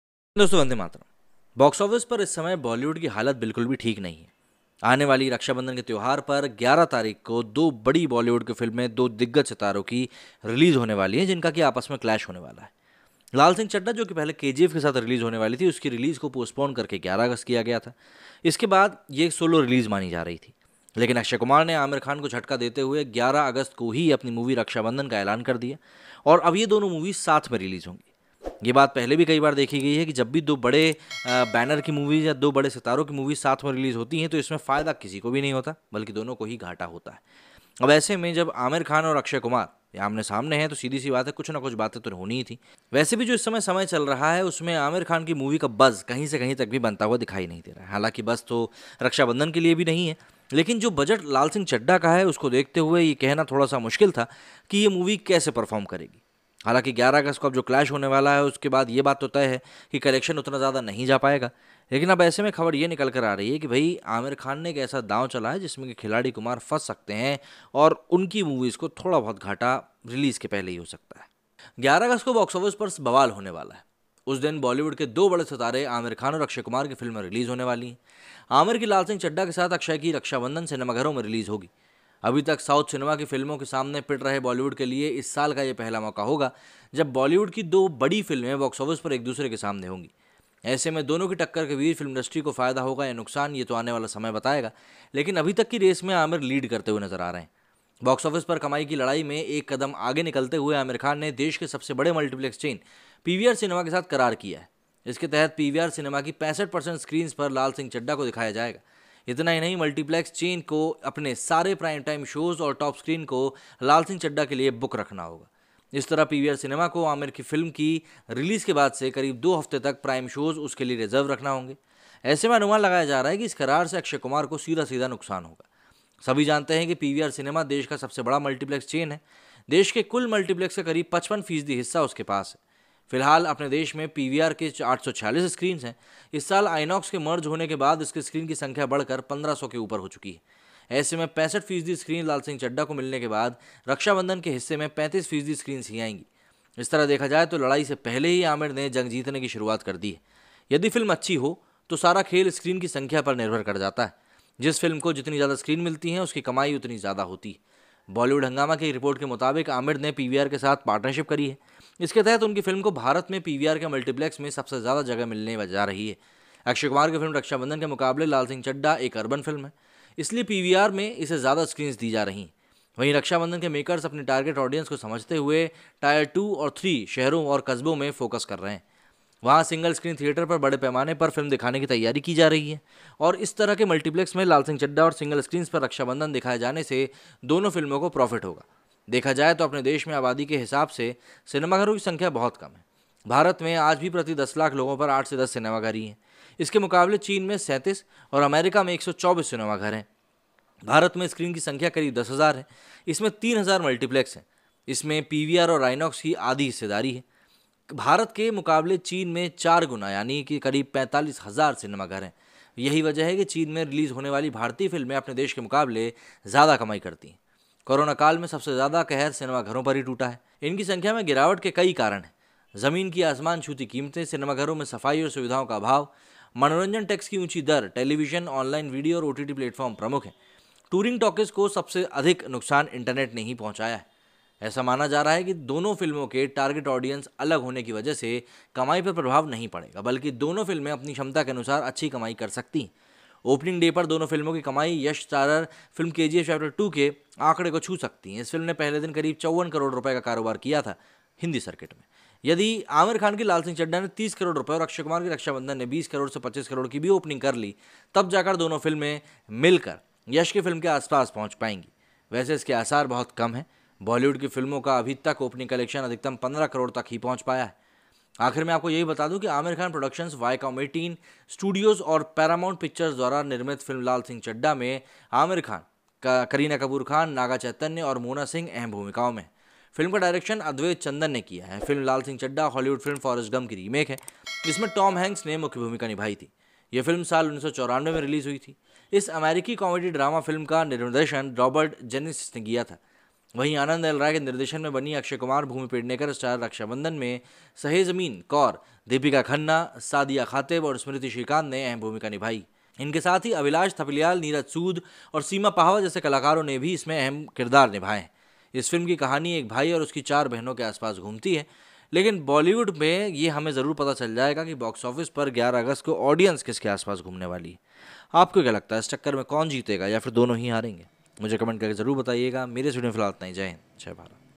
Laal Singh Chaddha Vs Raksha Bandhan Aamir Khan Sign A Mega Deal With PVR May Upset Akshay Kumar
Stay Blessed